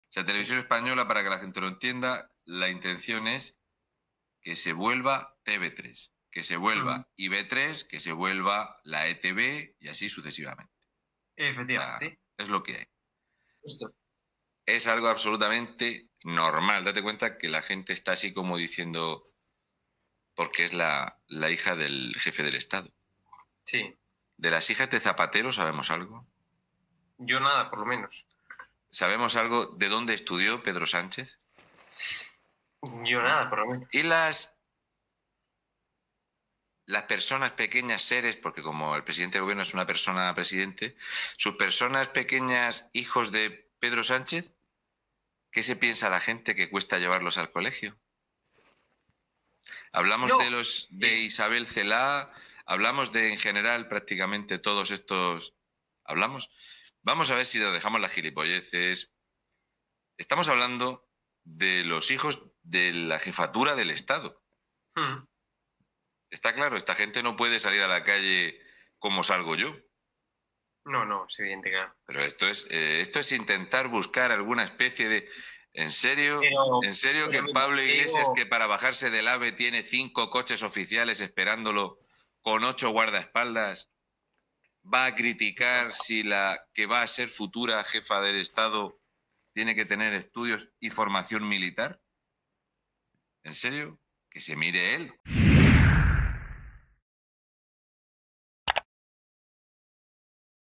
Un MURCIANO ENCABRONAO y TVE
Un murciano encabronao carga contra RTVE por el ataque que perpetraron con uno de sus rótulos hacia la princesa Leonor y se pregunta porque ninguna televisión habla de lo que cuesta el colegio de los hijos de otros líderes del Estado.

🔴 Descubre nuestra NUEVA web estadodealarmatv.es